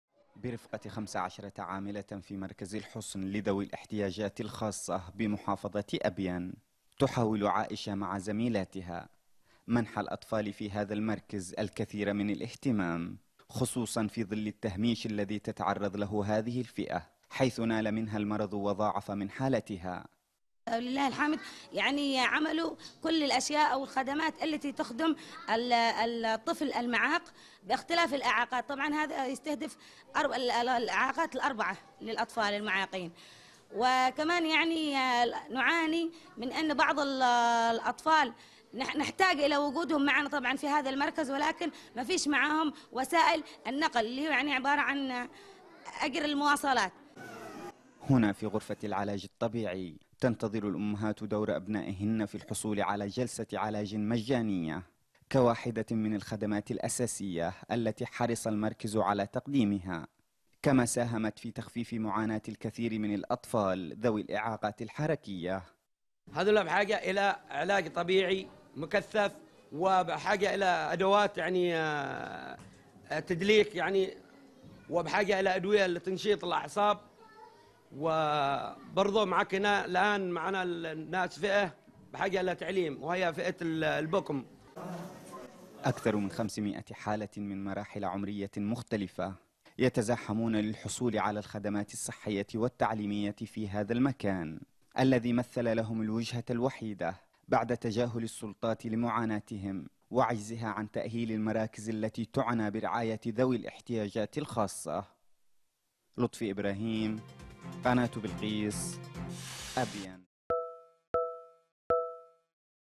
مركز صحي في أبين يقدم خدمات لأكثر من 500 حالة من ذوي الاحتياجات الخاصة| تقرير: لطفي إبراهيم
في أبين.. يعد مركز الحصن لذوي الاحتياجات الخاصة الوجه الوحيدة في المحافظة المعنية بتقديم الخدمات الصحية والتعليمية لأكثر من خمسمائة حالة.
واتهم رئيس المركز مفيد صالح الجهات الحكومية بتجاهل مطالب تأهيل المركز وتوفير المساعدات العلاجية والتعليمية لنسبة كبيرة من ذوي الاحتياجات الخاصة...